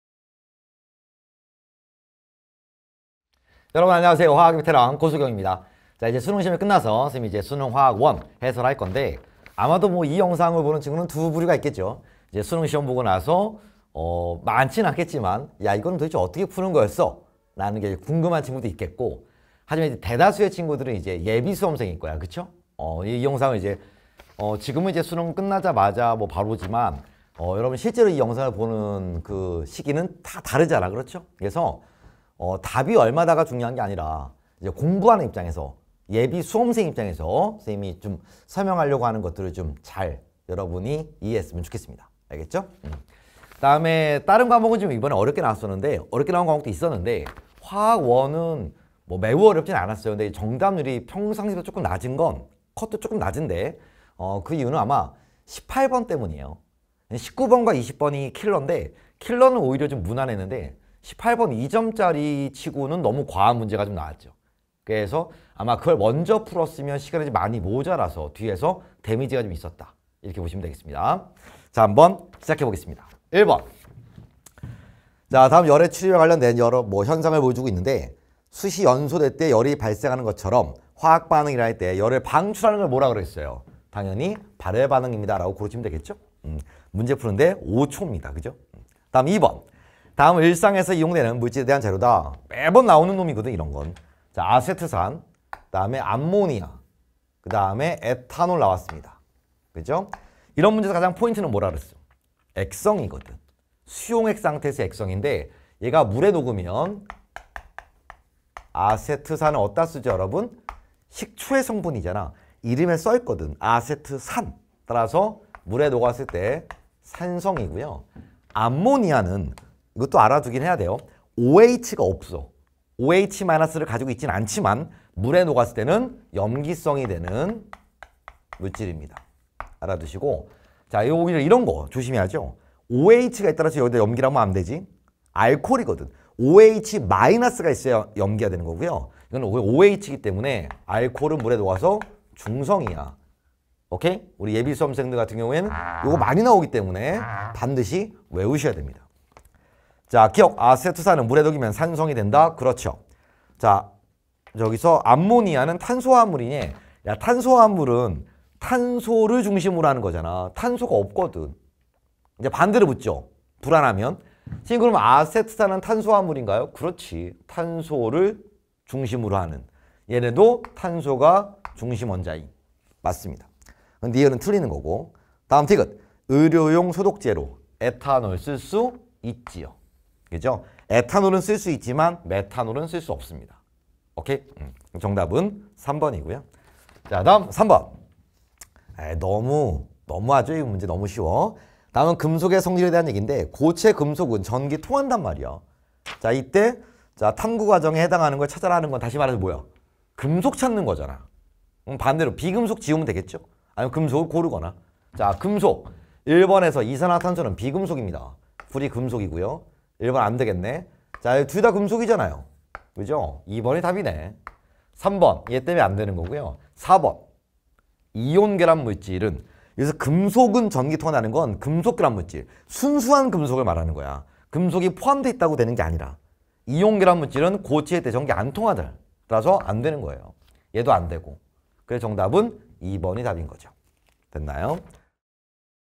[메가스터디] 화학 고석용쌤 - 2022학년도 수능 화학l 해설강의
고석용 선생님과 함께하는 2022학년도 수능 화학l 해설강의
자세한 내용은 메가스터디 홈페이지에서 확인해 주세요!

- 화학 고석용 선생님에 대해 더 자세히 알고 싶다면?